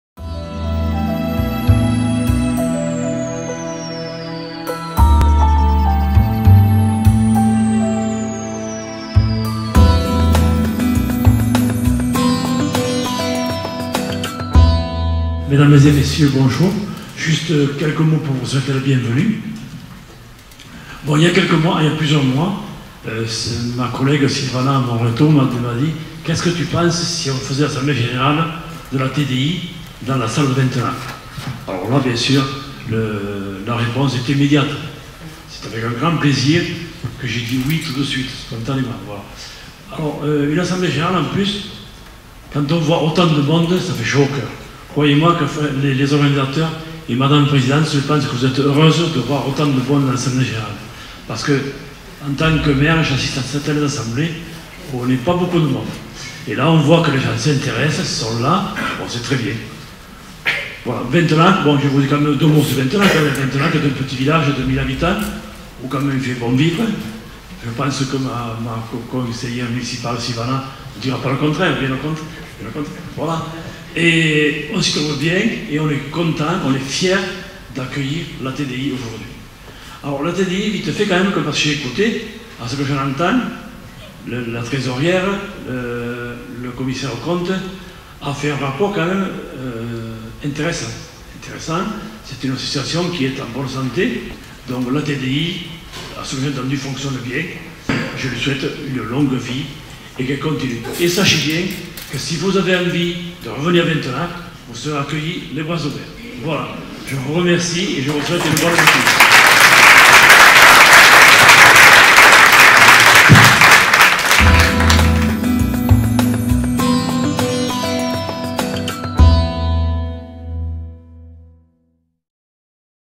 ATDI 2019 - 1er épisode - accueil par M. le Maire de Ventenac Cabardès
Les rencontres 2019 de l'ATDI ont expérimenté une formule originale.
Sur une matinée, un "espace éthique" a été créé dans la salle des fêtes du village de Ventenac Cabardès dans l'Aude.
Il a permis aux professionnels, institutions et bénéficiaires, d'échanger, sans a priori, autour des questions éthiques qui se posent dans l'accompagnement des personnes vulnérables.
9 épisodes pour faire le tour de cette riche matinée, revoir ou découvrir les contributions de chacun à cette réflexion essentielle.